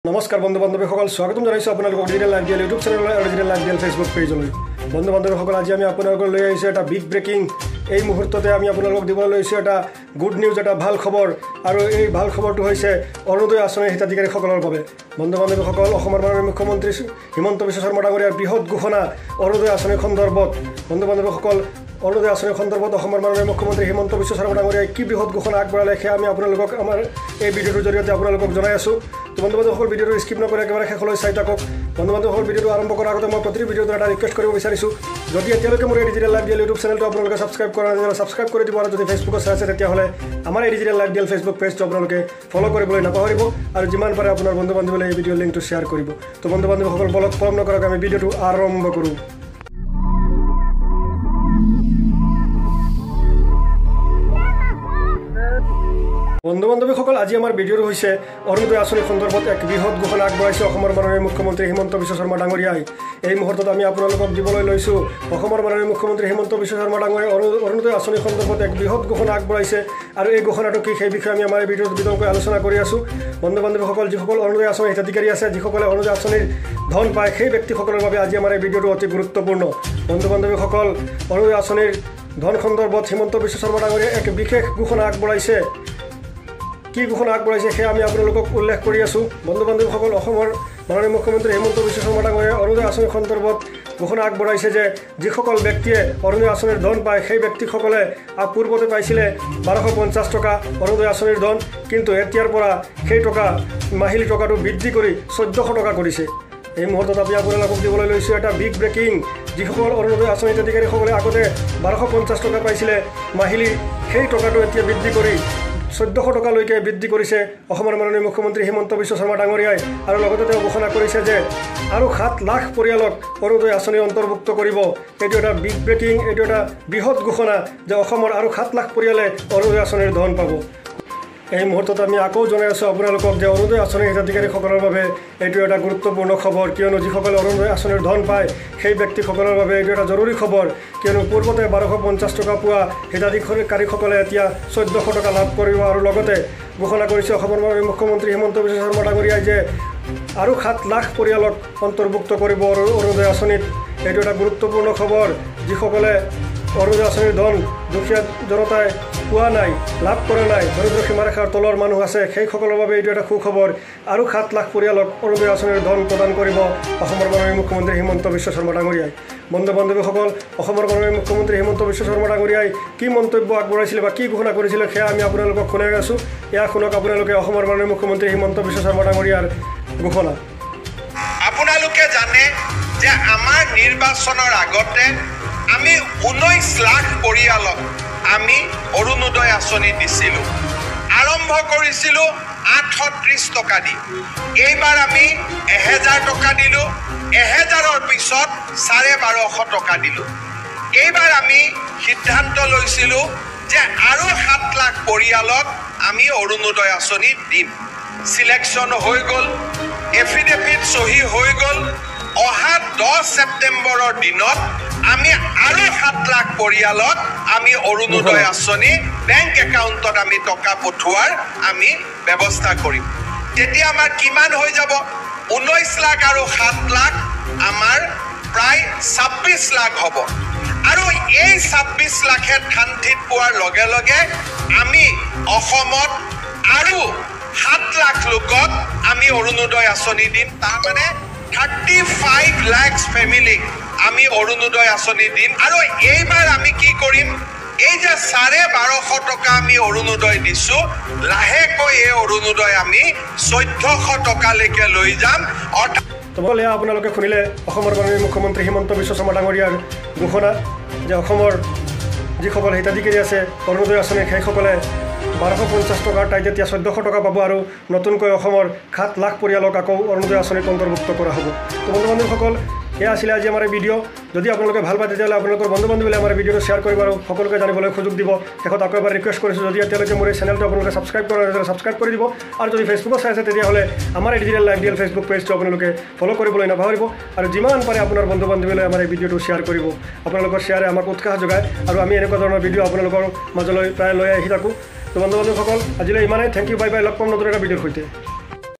ORUNODOI আচনি সন্দৰ্ভত হিমন্ত বিশ্ব শৰ্মাৰ বৃহৎ ঘোষণা , মাহিলী টকা বৃদ্ধি ।
নমস্কাৰ বন্ধুসকল, স্বাগতম জনাইছোঁ আপোনালোকক Digital Life DL channel লৈ । আজি আমি আপোনালোকলৈ লৈ আহিছো ORUNODOI আচনি সন্দৰ্ভত হিমন্ত বিশ্ব শৰ্মাৰ বৃহৎ ঘোষণা , মাহিলী টকা বৃদ্ধি । ‎@digitallifedl5755  ।সবিশেষ জানিবলৈ আমাৰ VIDEOটো শেষলৈকে চাওক। digital_life_dl @digitallifedl5755